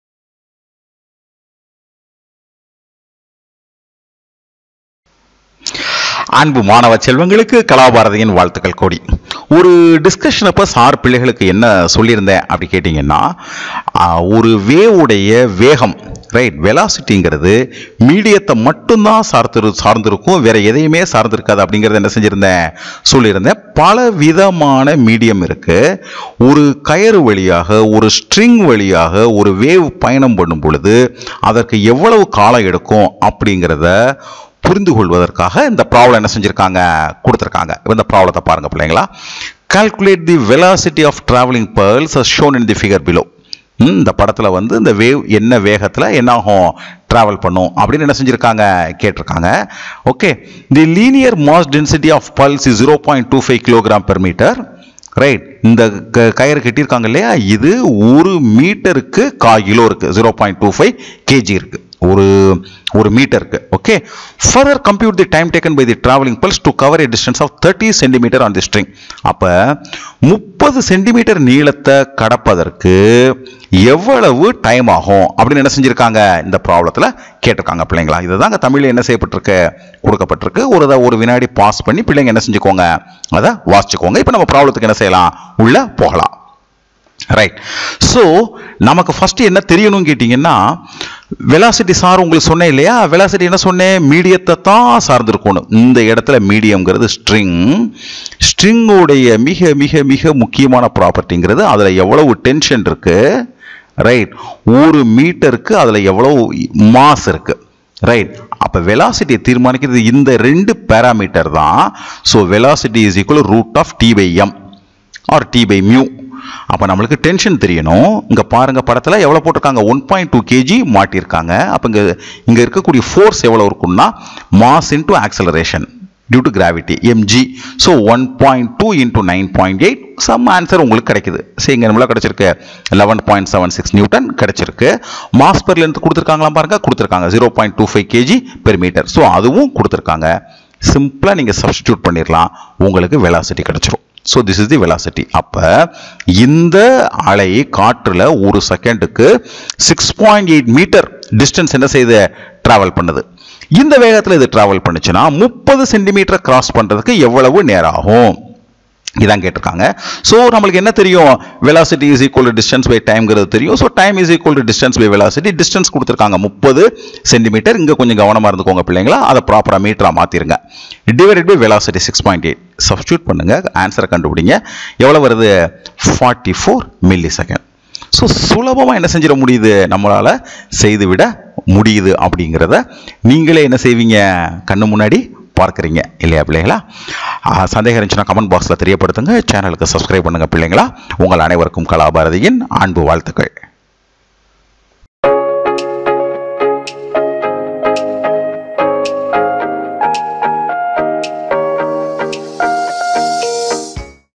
6.Physics | Waves | Example 11.6
Calculate the velocity of the travelling pulse as shown in the figure  below. The linear mass density of pulse is 0.25 kg m-1. Further, compute the time taken by the travelling pulse to cover a distance of 30 cm on the string.